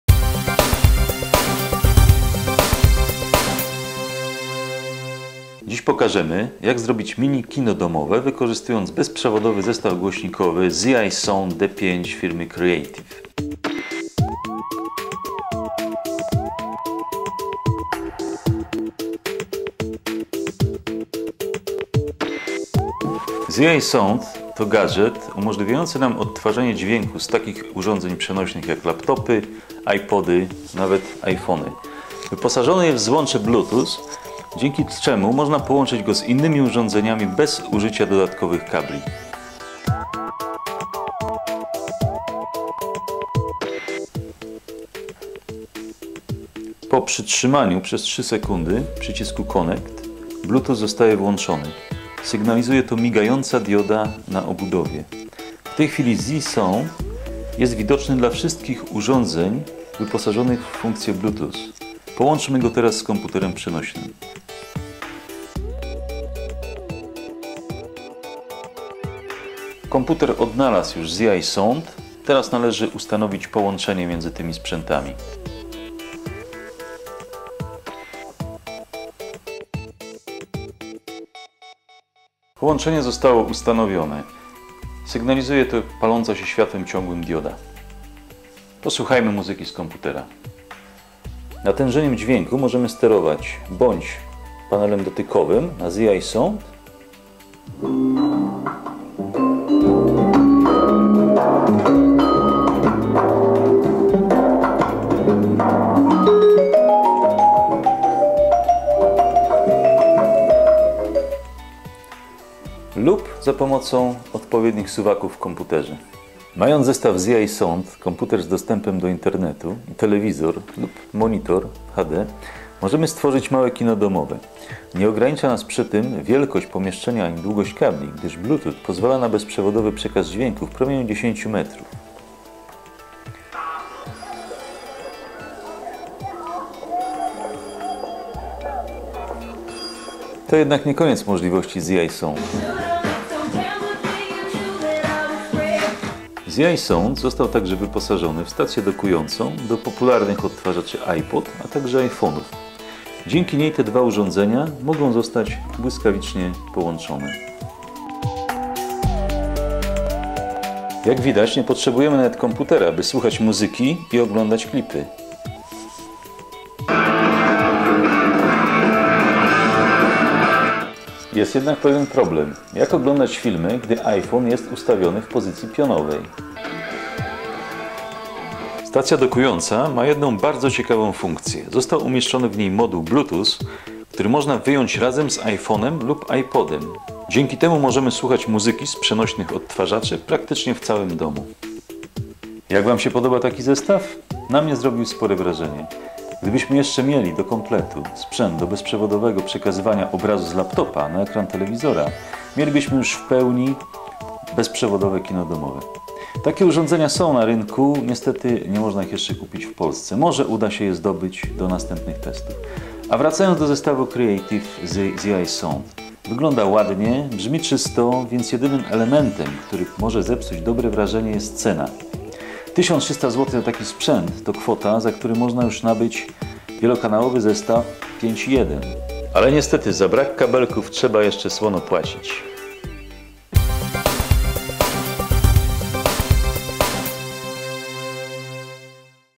Jak zrobić mini-kino domowe wykorzystując bezprzewodowe głośniki?
Prezentujemy gadżet firmy Creative: bezprzewodowy zestaw głośnikowy Zii Sound D5, przez który można odtwarzać dźwięk z każdego urządzenia posiadającego moduł Bluetooth (komputery, telefony komórkowe), a także z iPodów i iPhonów. Film pokazuje, w jaki sposób można uzyskać efekt kina domowego, odtwarzając np. video z YouTube na ekranie cyfrowego telewizora, a dźwięk na bezprzewodowych głośnikach Creative.